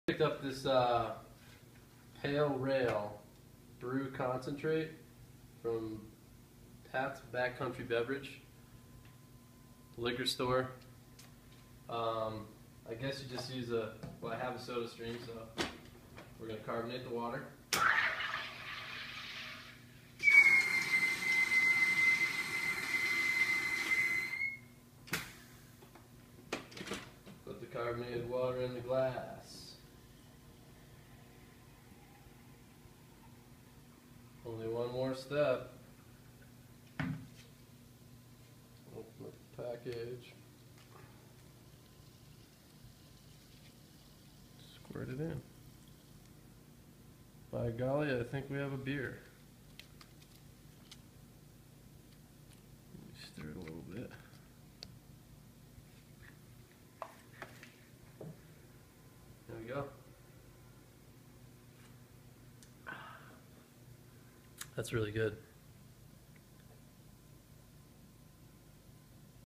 How to Make a Beer with A SodaStream
I bought a pack of Pats Backcounty "Brew concentrate" from Argonaut liquors in Denver and decided to shoot a video on how it works. WHAT did NASA come up with this!!? The concentrate taste and drinks exactly like a beer its wild you have to try it to believe it!!!